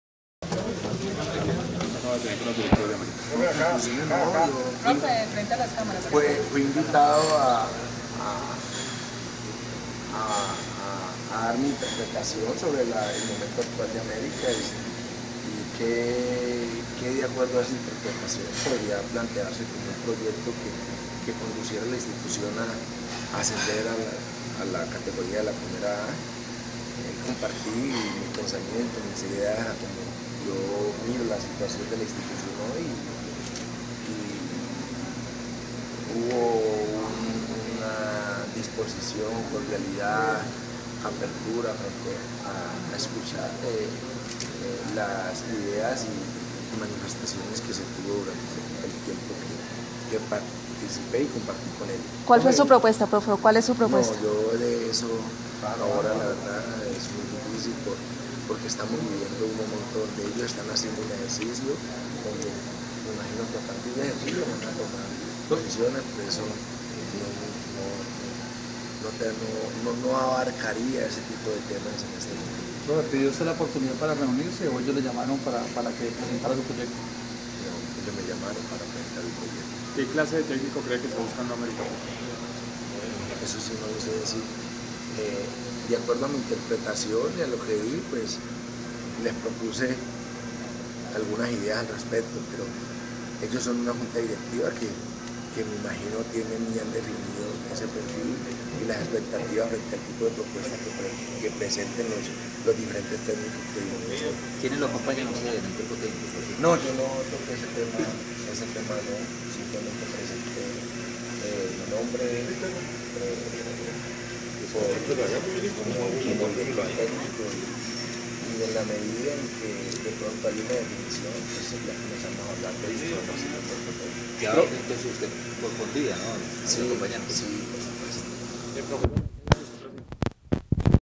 "Me pidieron presentar proyecto" William Libreros
William Libreros también presentó su proyecto para eer DT del América